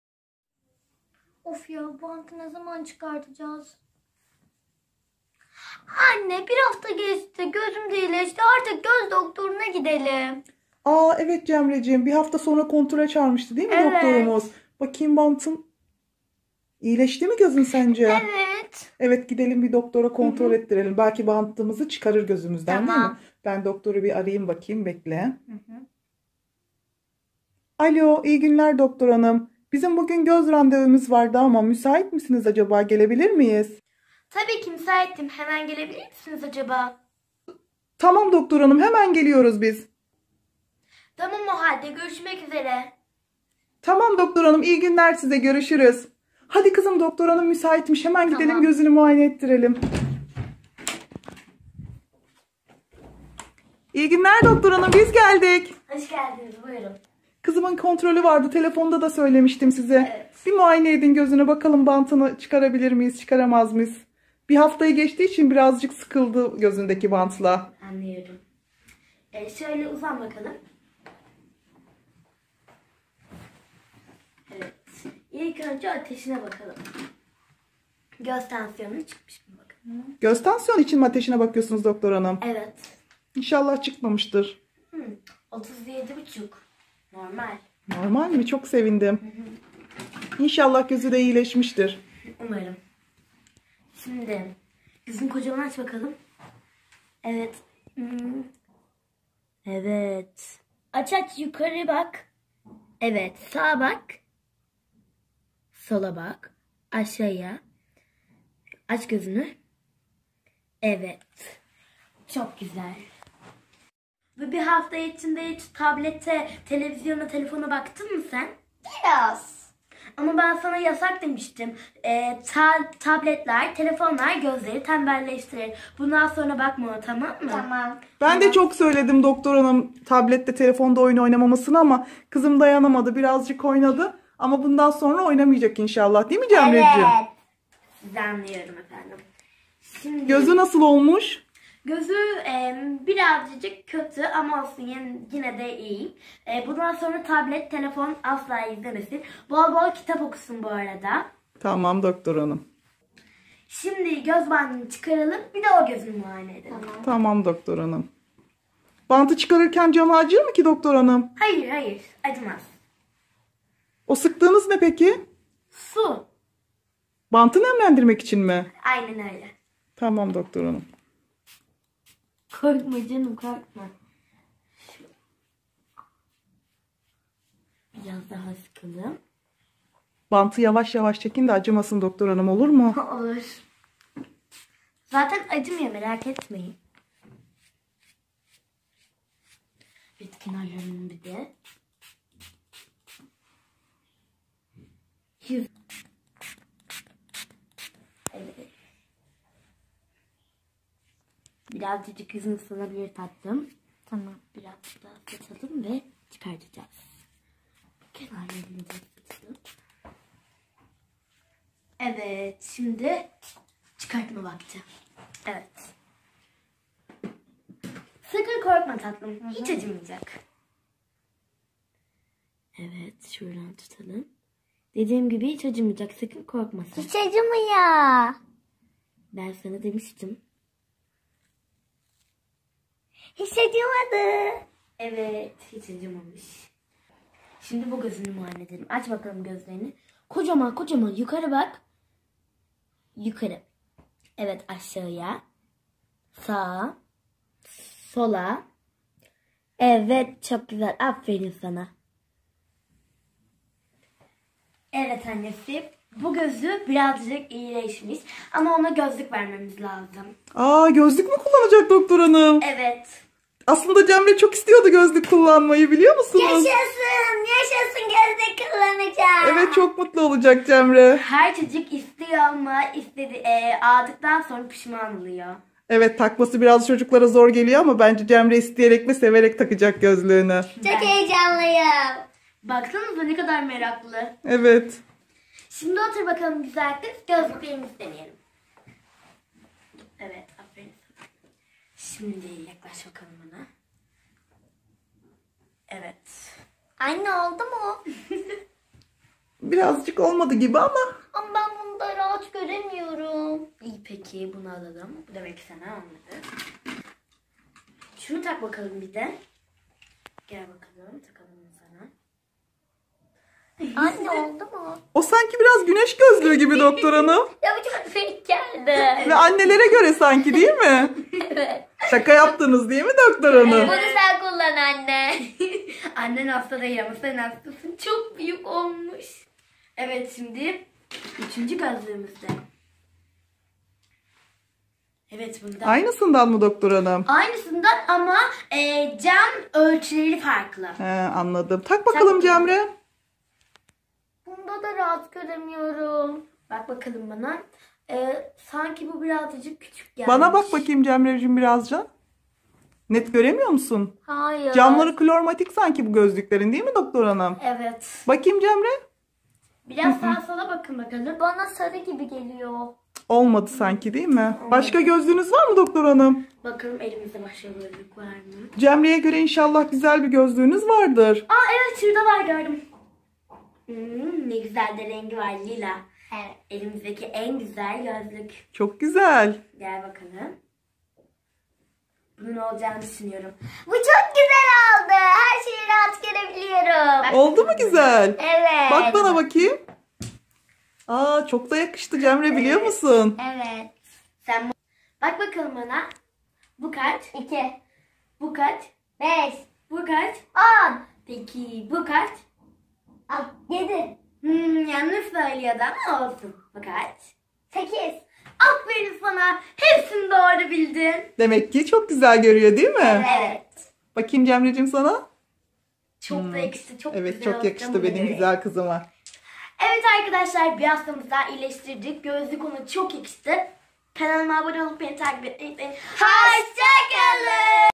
Doktorculuk Oyunu|Cemre ile Doktor Oyuncak Seti ile Doktorculuk Oynadık |Zümra Nur Göz Doktoru Oldu
Oyuncak doktor seti ile doktorculuk oyunu oynadık ben göz doktoru oldum cemreyi muayene ettim ve ona gözlük verdim